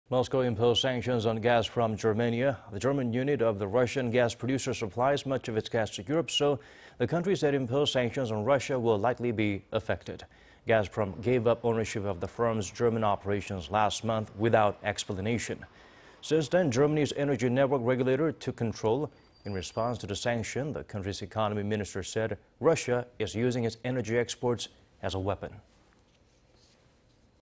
Russia imposes sanctions on German unit of Gazprom
러, 가스프롬 독일 법안 가스 공급 중단

Moscow imposed sanctions on Gazprom Germania.
The German unit of the Russian gas producer supplies much of its gas to Europe, so the countries that imposed sanctions on Russia will likely be affected.
Gazprom gave up ownership of the firm's German operations last month without explanation.
Since then, Germany's energy network regulator took control.
In response to the sanction,... the country's Economy Minister said Russia is using its energy exports as a weapon.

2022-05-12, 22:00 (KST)